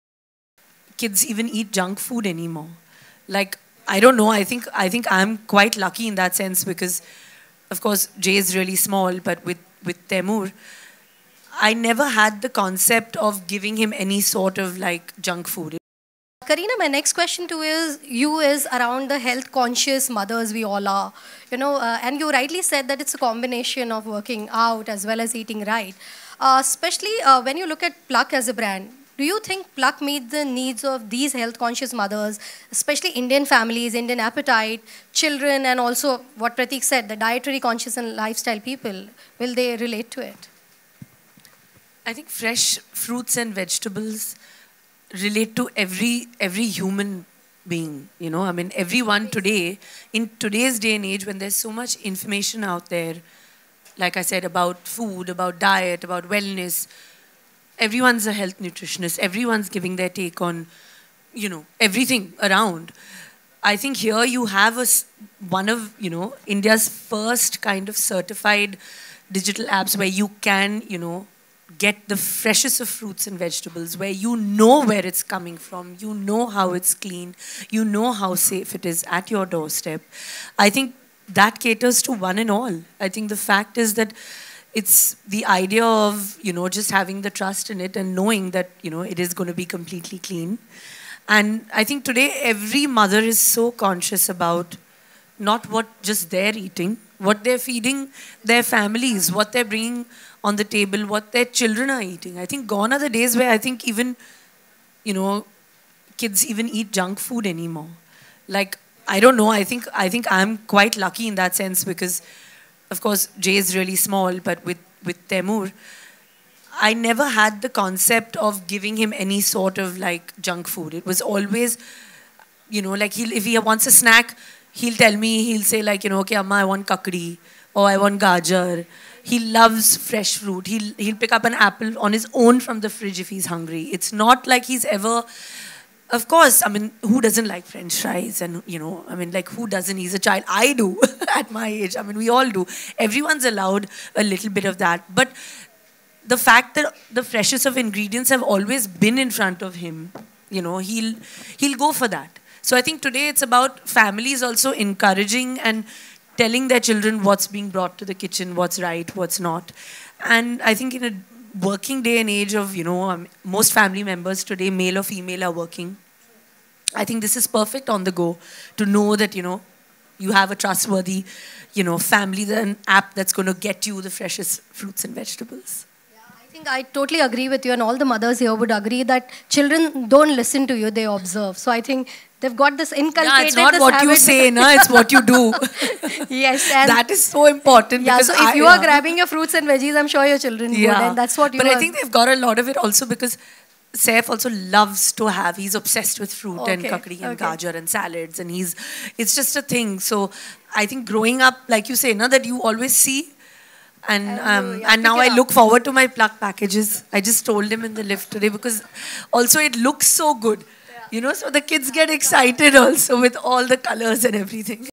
Kareena Kapoor Reveals She Doesn't Give Junk Food To Her Kids
Kareena Kapoor reveals the eating habits of son Taimur Ali Khan and says that she doesn't feed him junk food.